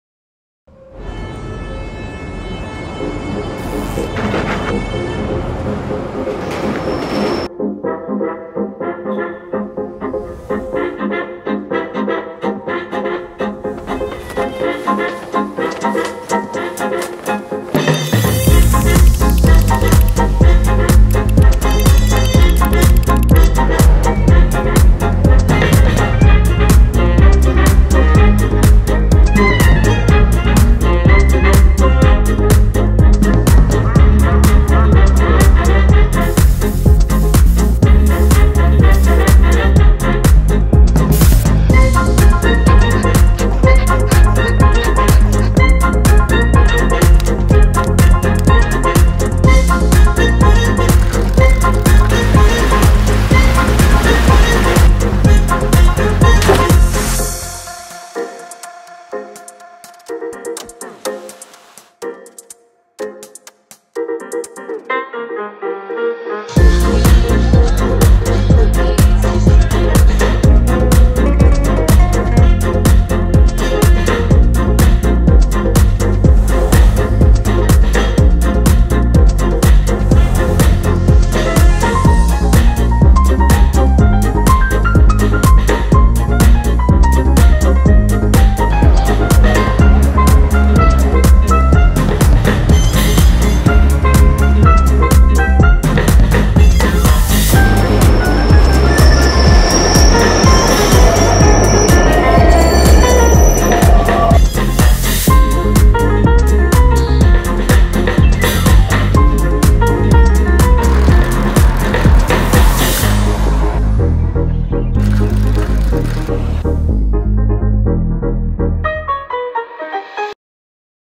a City of Smiles | Aarhus, Denmark | CINEMATIC TRAVEL FILM
Aarhus city, the 2nd largest city in Denmark, previously the culture capital of Europe and known by it's people as the city of smiles. This is my first attempt at making a travel video to this extent. Let me know what you think.

Shot on Canon R6, Handheld & on a Gimbal